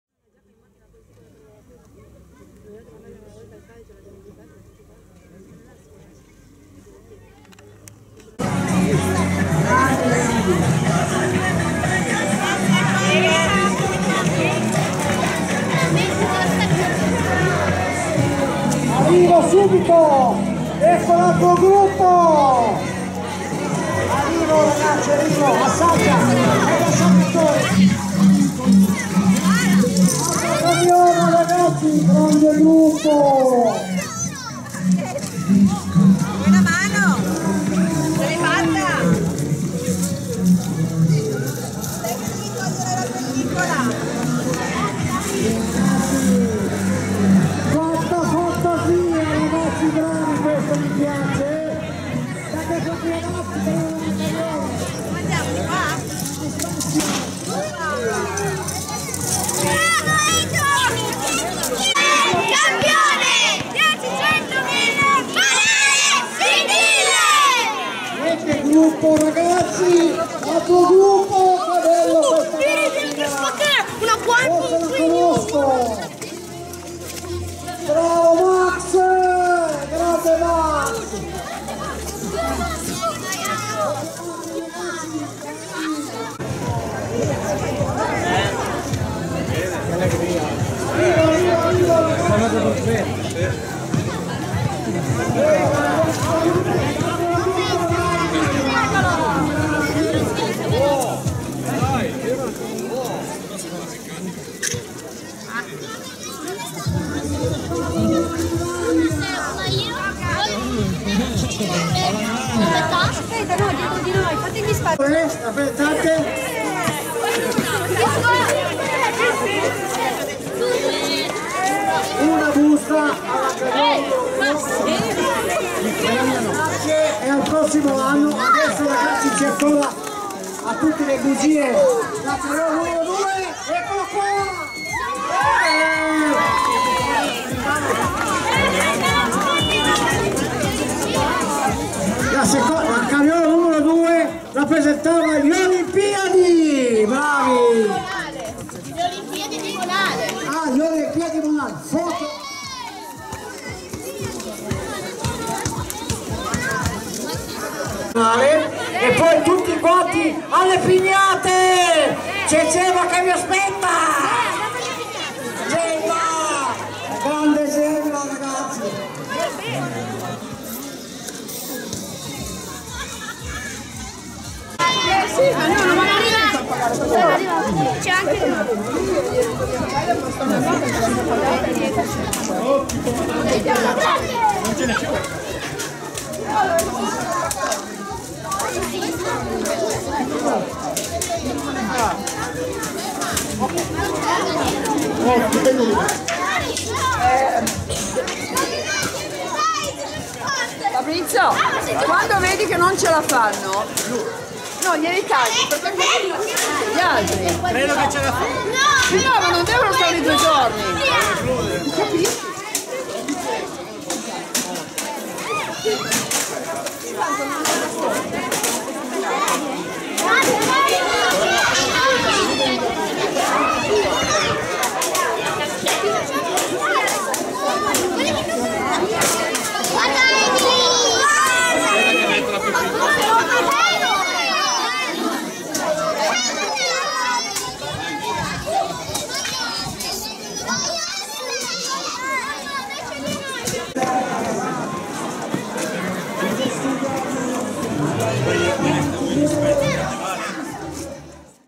Carnevale in Carriola, Monale, Sfilata Carriole e Gruppi Mascherati
Allegro Carnevale in Carriola, sfilata e premiazione Carriole e Gruppi Mascherati, rottura Pignatte, distribuzione Bugie, Monale(AT). 2-3-2024